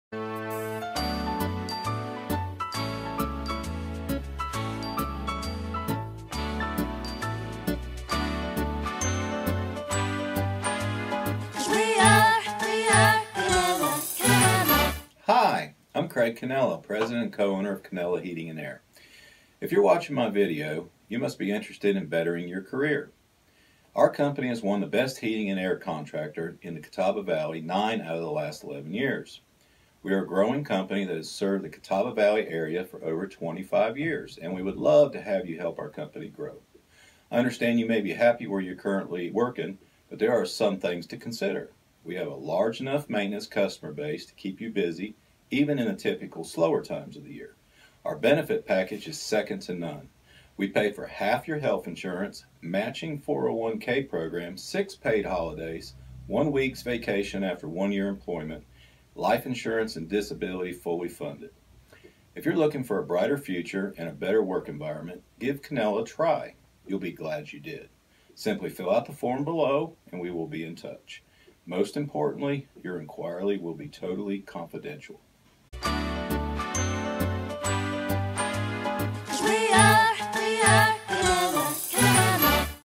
Better Your HVAC Career with Canella Heating and Air Conditioning
If you are looking for a better career in heating and air conditioning in the Hickory and Greater Catawba Valley area give Canella a try!